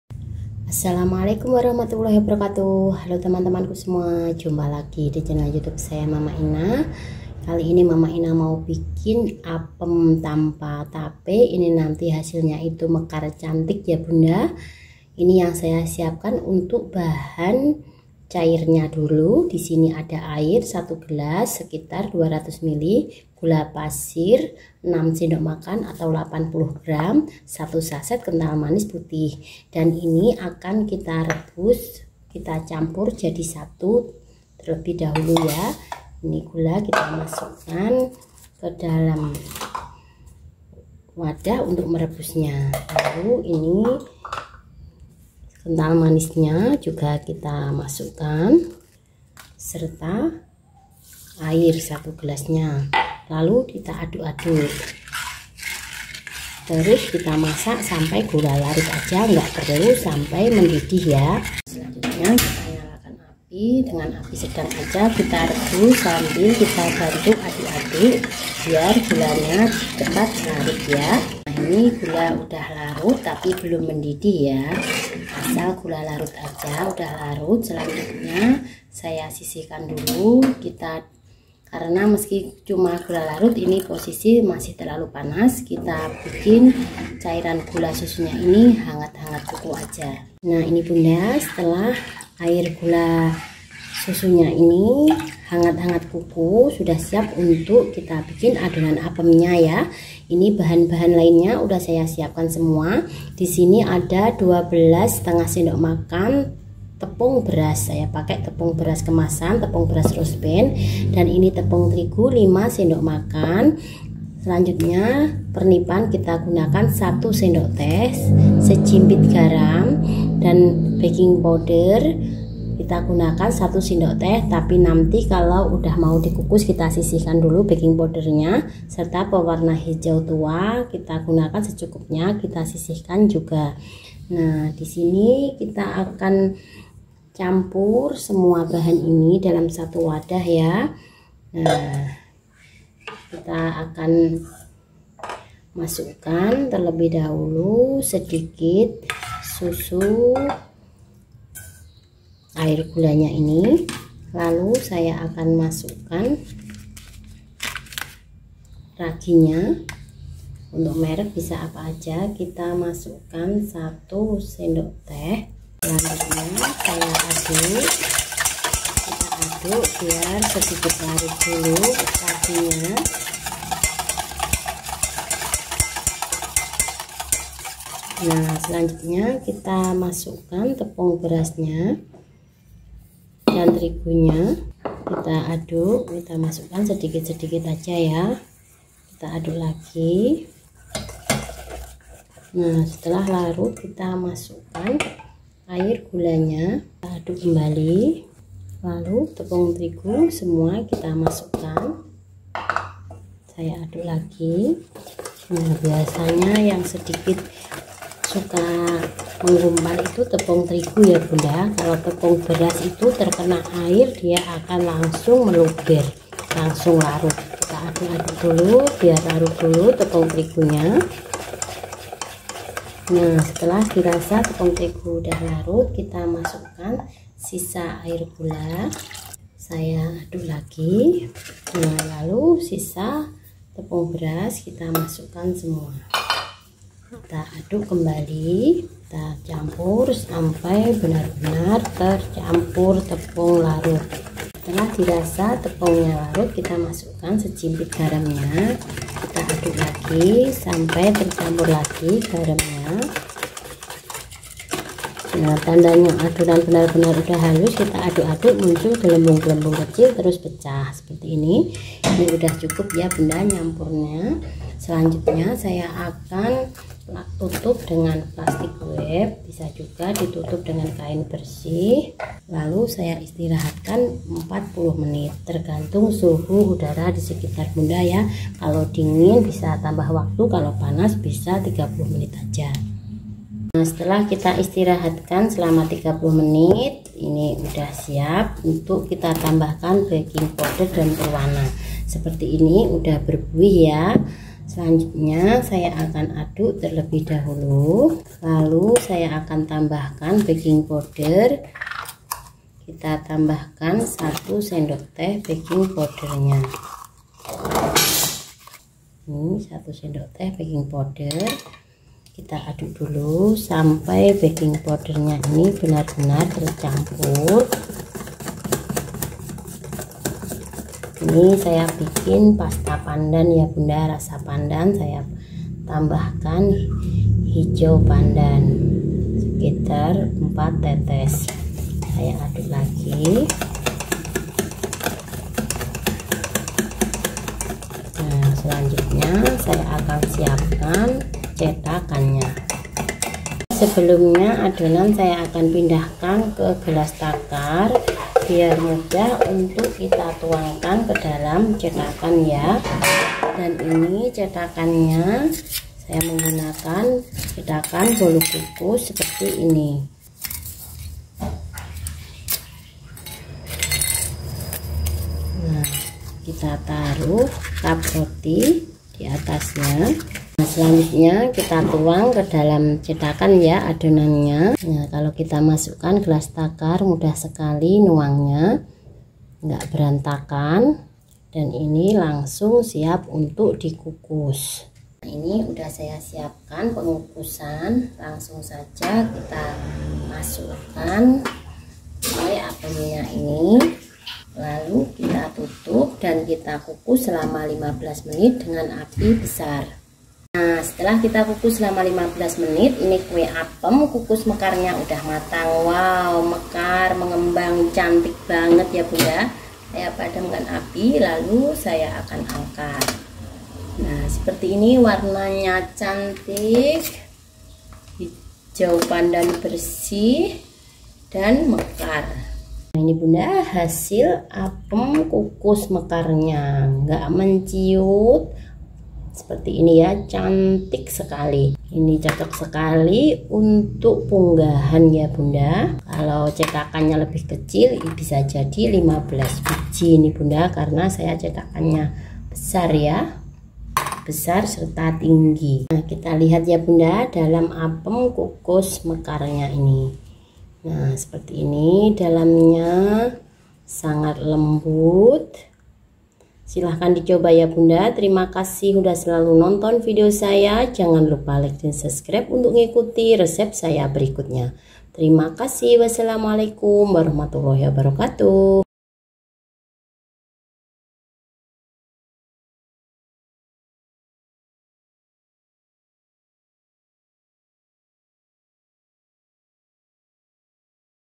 tips paling mudah buat apem kukus tanpa tape mekar anti bantat
tips paling mudah buat apem tanpa tape mekar anti bantat

bahan bahan :
125 gr tepung beras/12,5 sdm
5 sdm tepung terigu
6 sdm gula pasir/80 gr
1 saset kental manis
1 gelas air/200 ml
1 sdt ragi instan
1 sdt beking piuder
Sedikit garam pewarna hijau